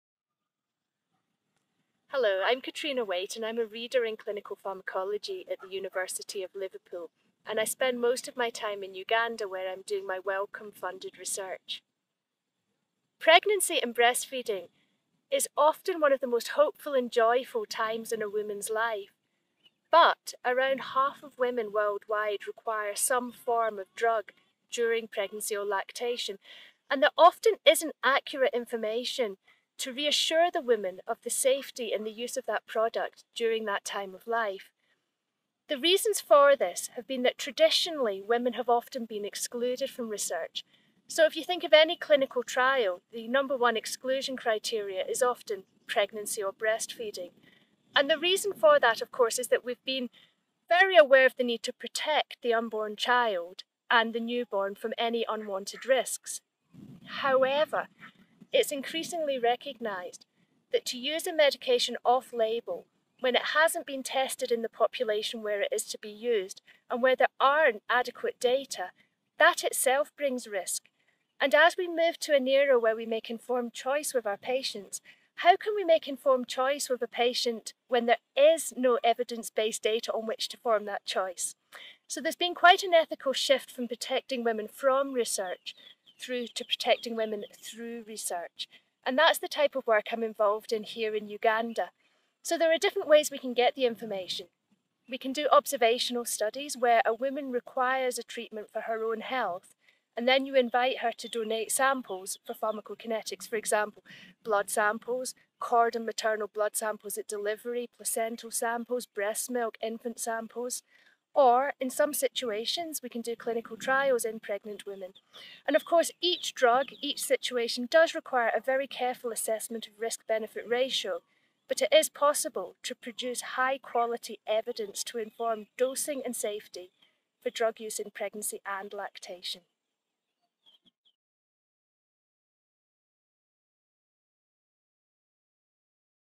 Research in pregnant women - Dr Catriona Waitt
Dr Catriona Waitt discusses research in pregnant women. Dr Waitt, Reader in Clinical Pharmacology at the University of Liverpool, studies HIV drugs in pregnant and breastfeeding mothers and their infants. Her unit in Uganda was involved in supporting the response to the dolutegravir safety alert.